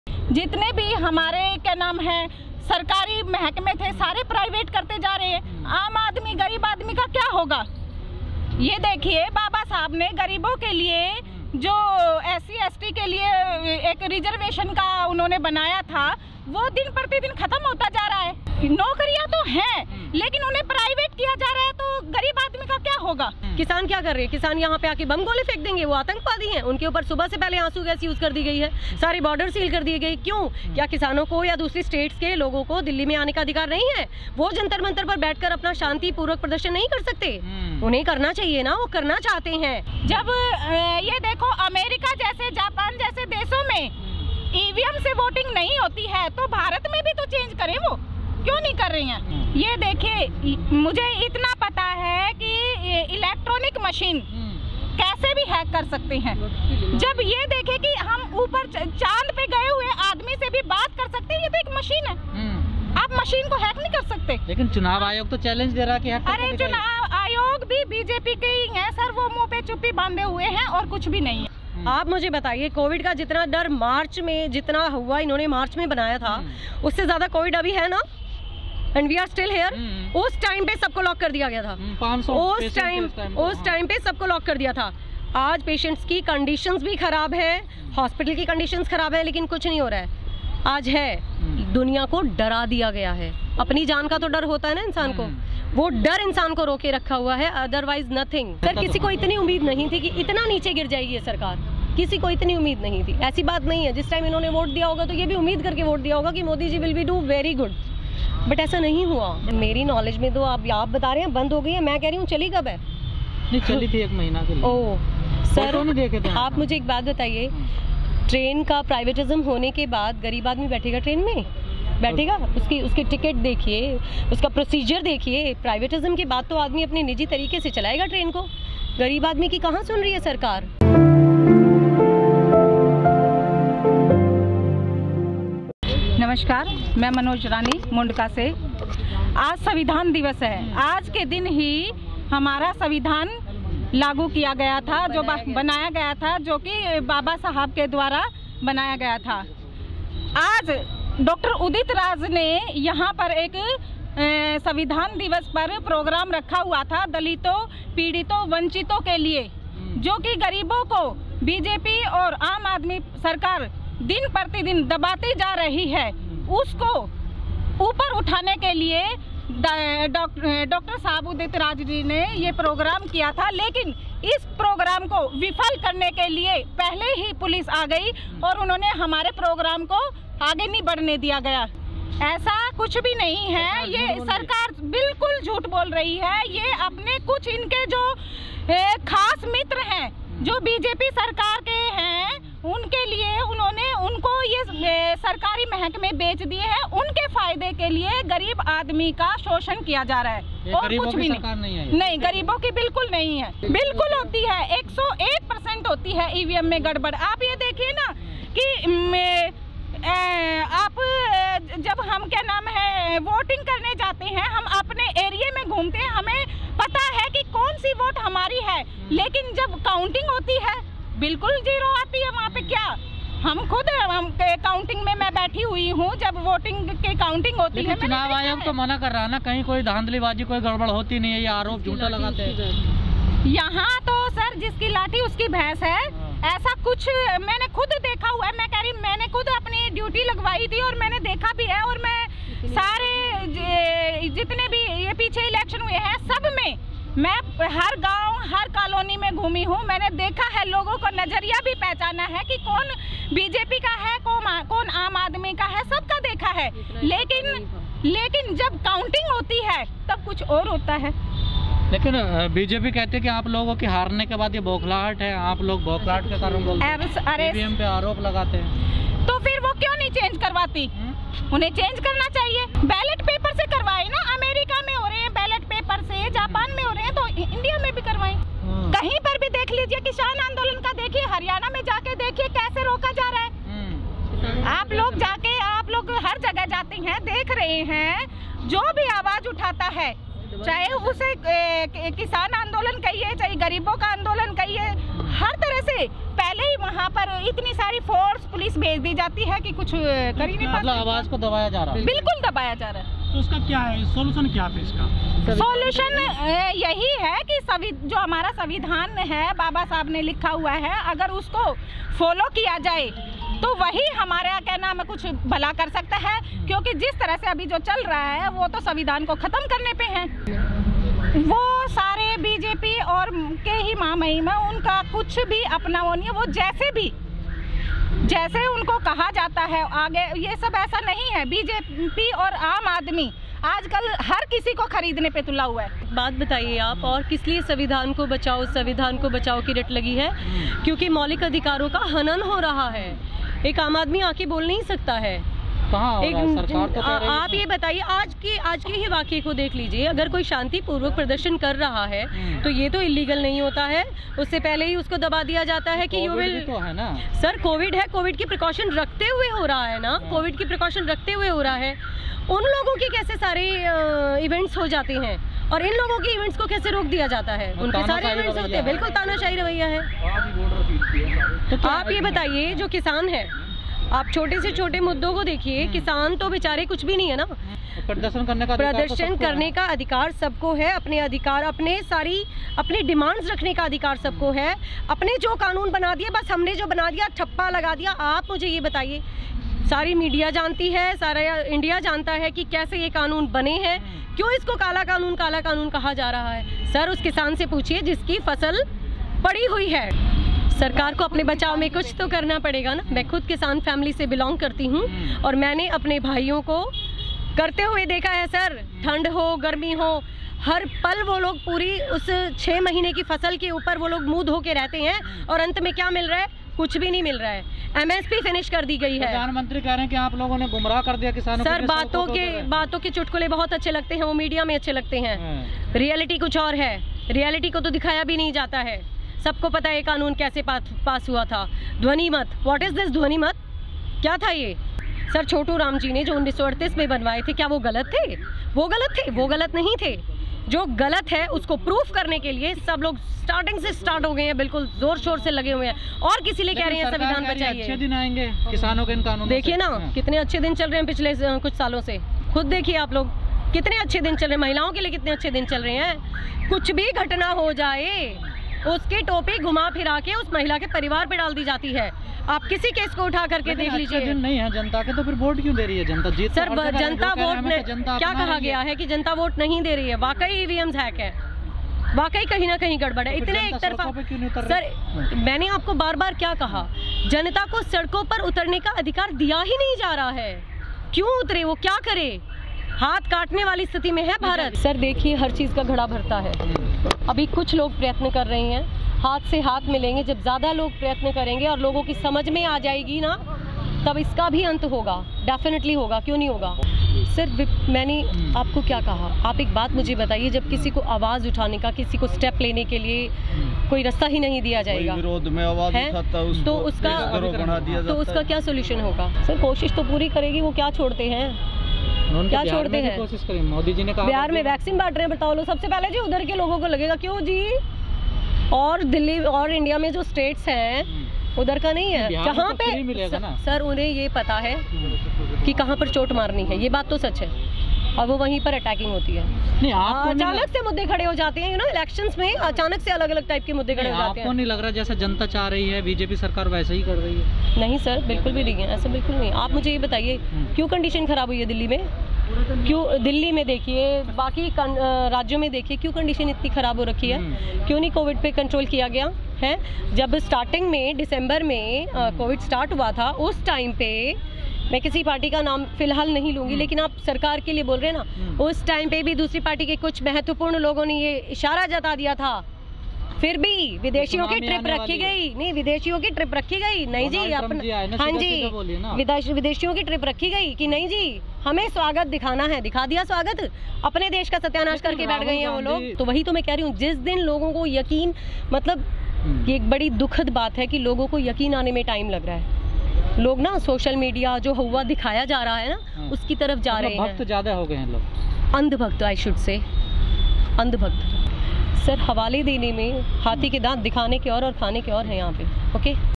बाप रे! अंग्रेजी बोलने वालीं दलित महिलाओं ने खोली मोदी की पोल!
ईवीएम हेराफेरी, निजीकरण, दलितों पर अत्याचार और सरकारी कंपनियों को बिकने के खिलाफ दलितों ने आज दिल्ली स्थित अंबेडकर भवन में प्रदर्शन किया। इस मौके पर दलित महिलाओं ने मोदी सरकार की नीतियों को खुलकर विरोध जताया और मोदी सरकार को तानाशाह सरकार करार दिया।